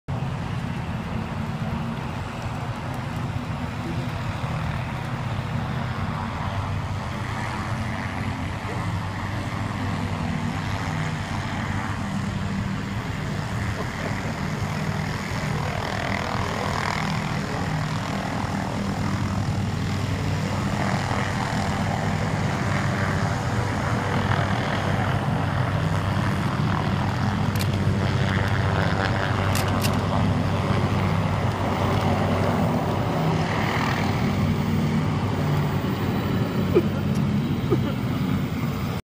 1/6th Scale Tiger Tanks
1/16th scale Tiger I Tanks at the Arsenalen Family Day